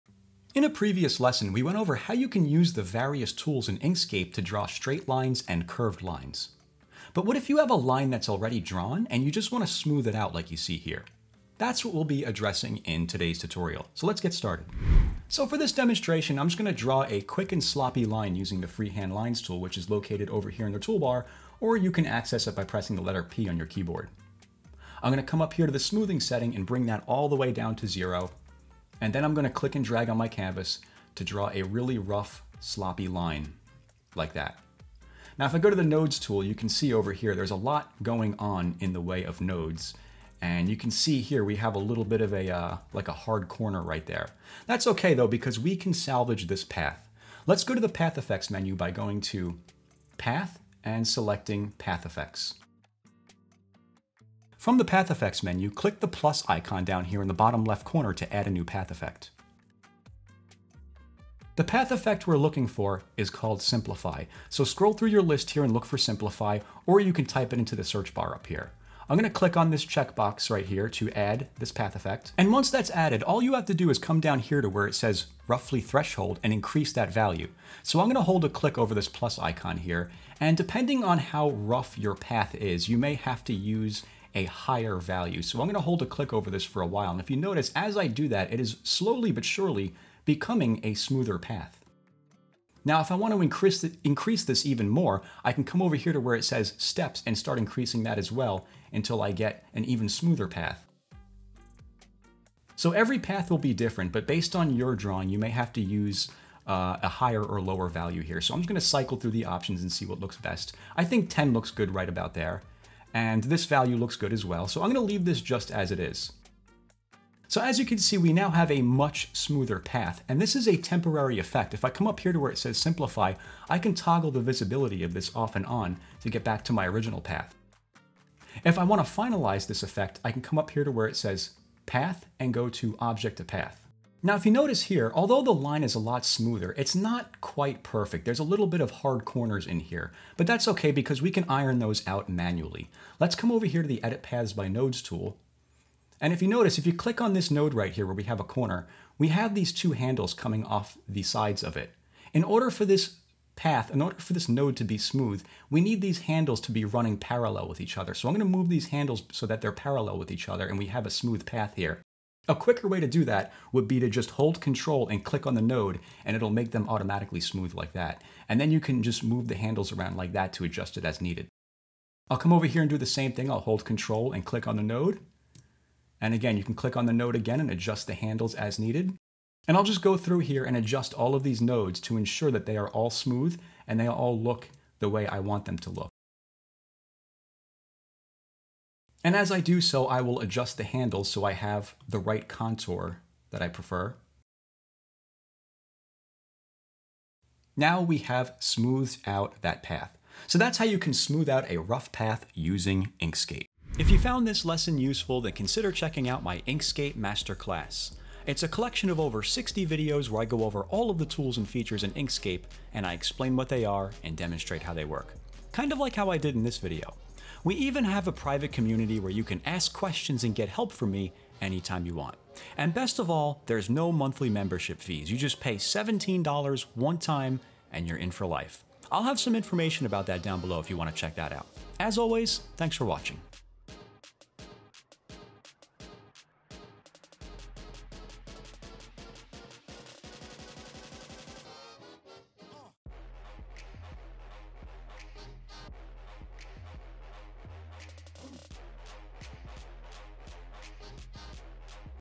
How To Smooth An Already Drawn Path In Inkscape
In a previous tutorial we went over how to draw smooth paths, but what if you want to smooth out a path that has already been drawn? That's what we'll be covering in this tutorial.

In short, this is accomplished using the "Smooth" path effect, which serves as an alternative to Illustrator's Smooth Tool.

🔗 RESOURCE LINKS

🎓 EXPLAINER COURSES

💬 SOCIALS

🎵 MUSIC USED